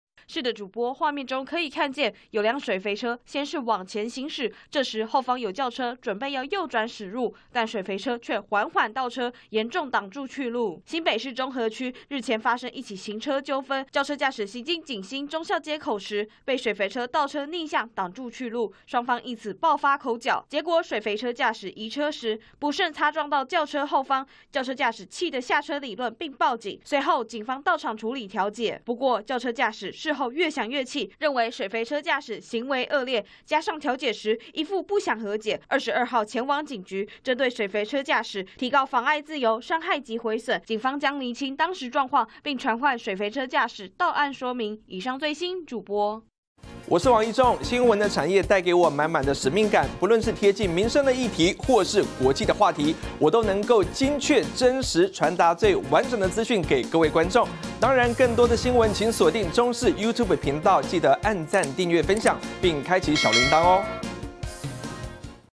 水肥車逆向擋路+擦撞 轎車駕駛調解不成怒提告│中視新聞 20221128
新北中和發生一起行車糾紛，有輛水肥車逆向倒車擋住轎車去路，移車時又不慎擦撞到轎車後方，轎車駕駛調解不成，氣得到警局提出告訴，連線記者鍾孟儒。